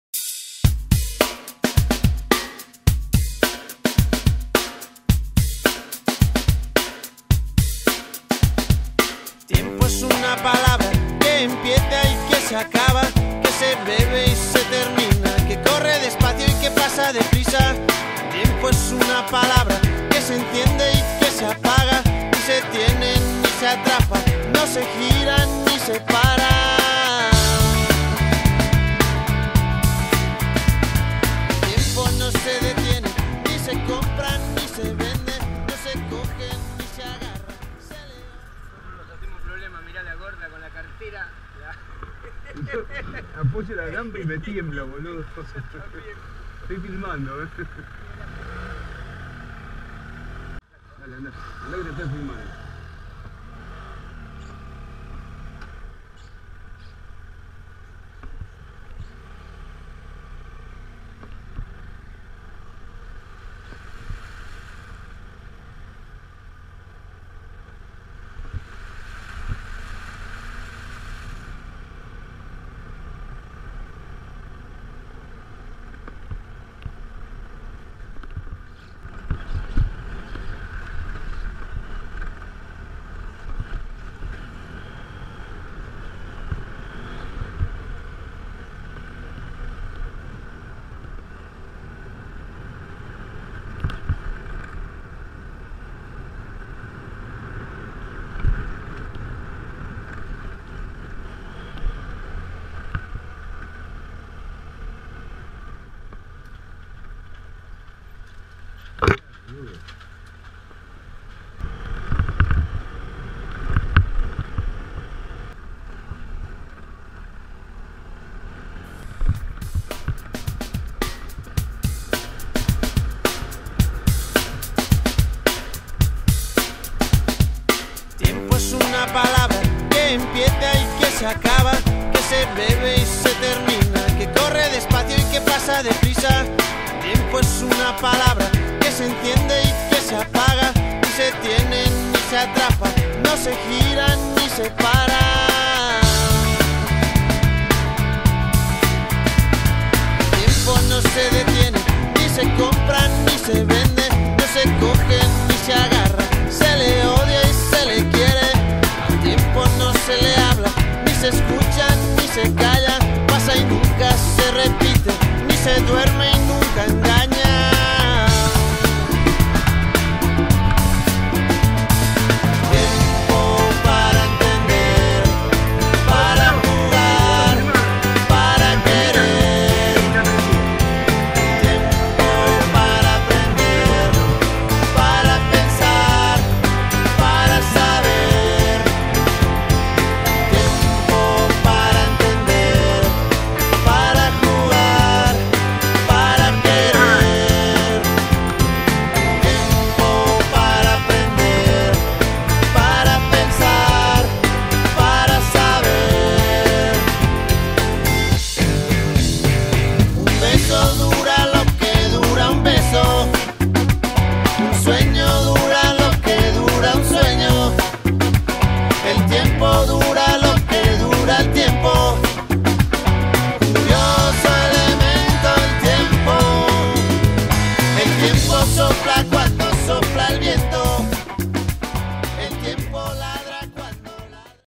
2014 01 04 motoqueada carilo entrada
una vueltita en moto, un poquito de barro, una linda mañana.